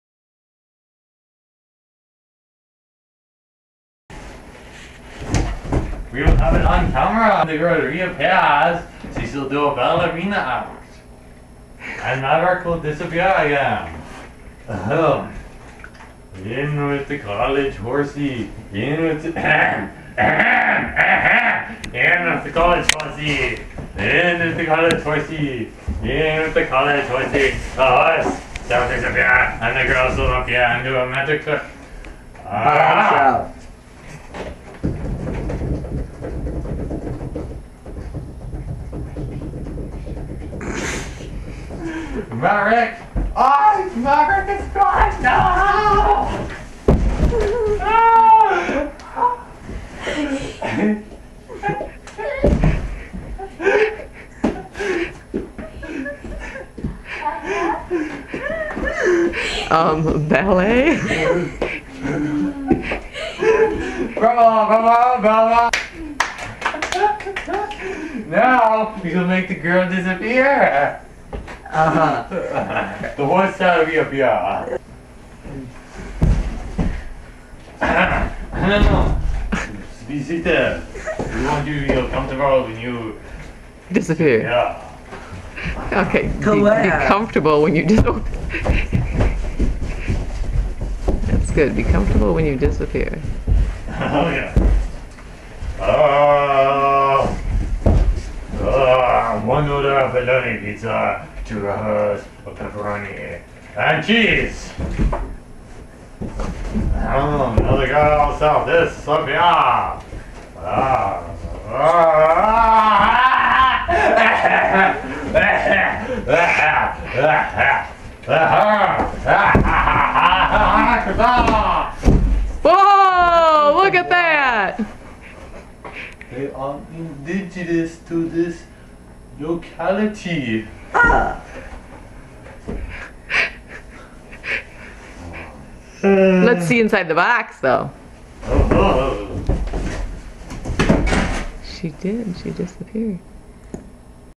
The Disappearing Girl
Mary and Levi take a break from getting-ready-to-move chores to have a little fun.